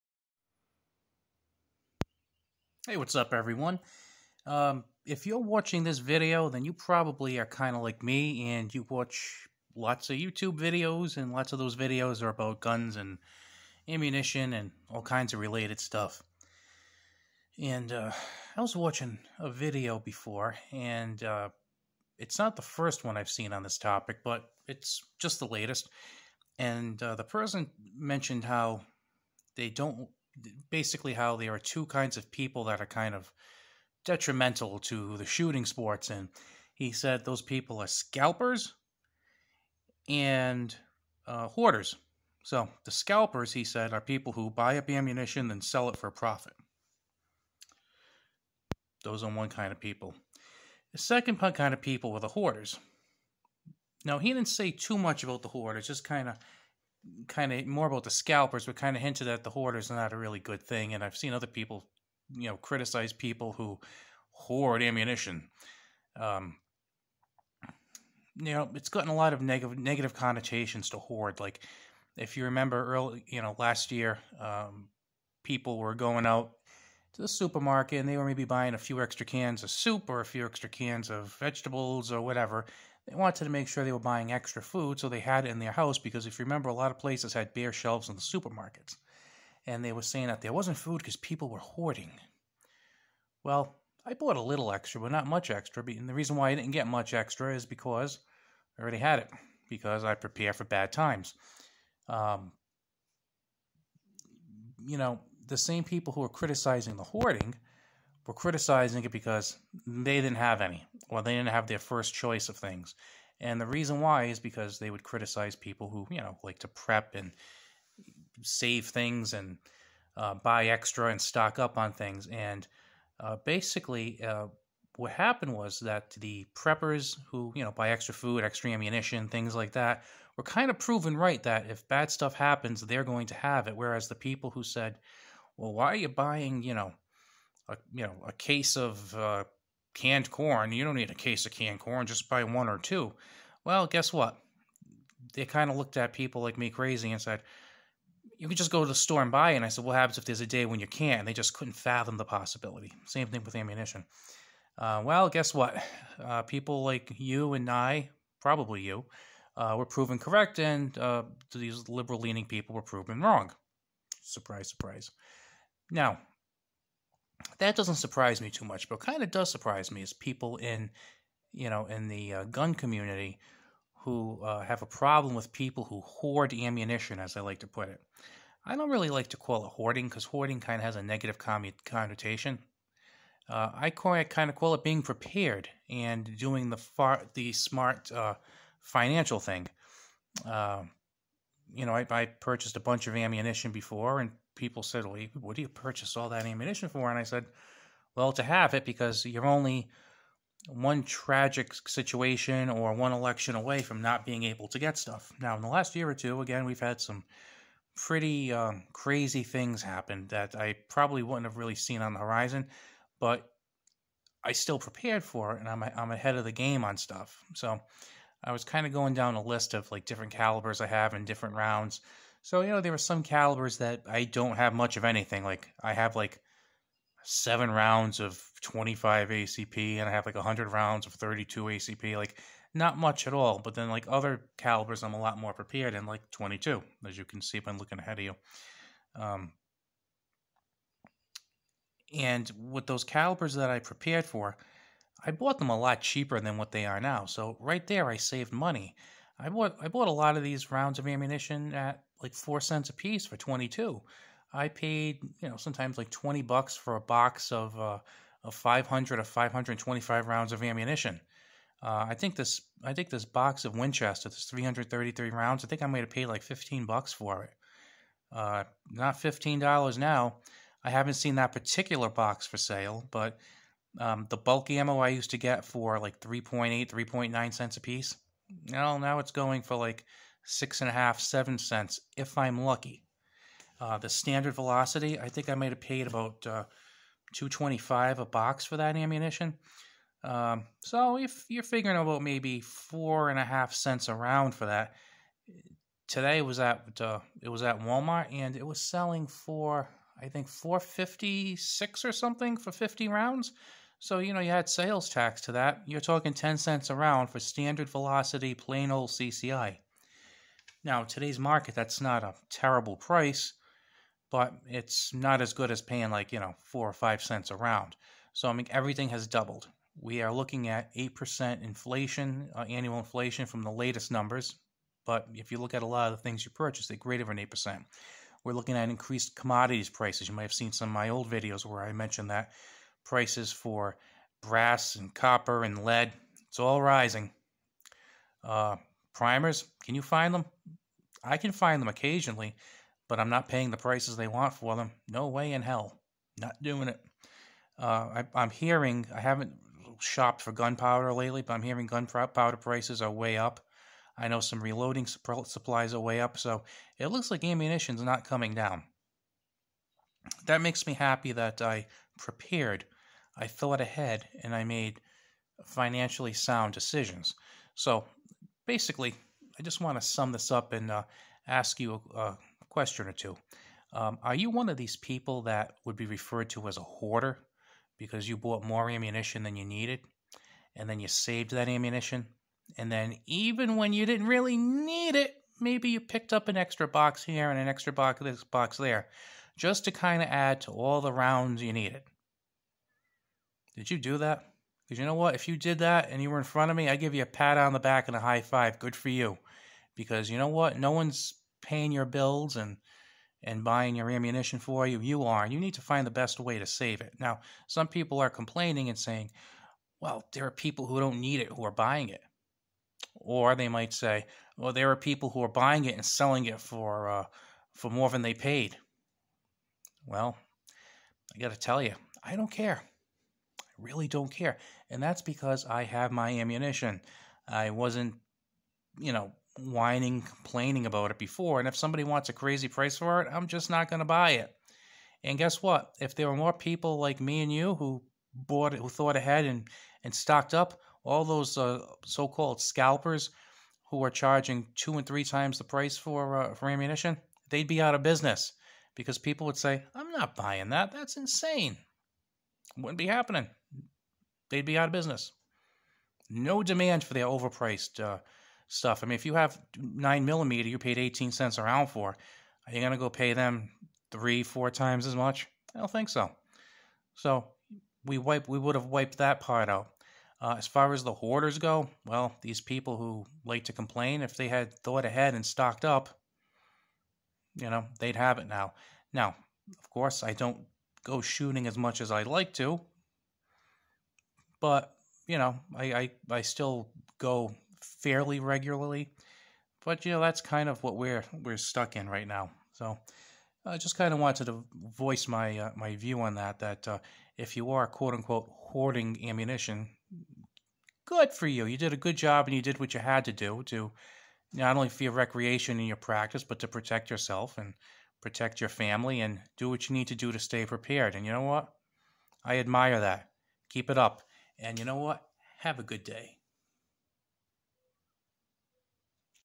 Do You Hoard Ammo?
Stocking up on important items is a good thing, a responsible thing.  Yet, there are those who are ill prepared who like to complain about others rather than take stock of their failures.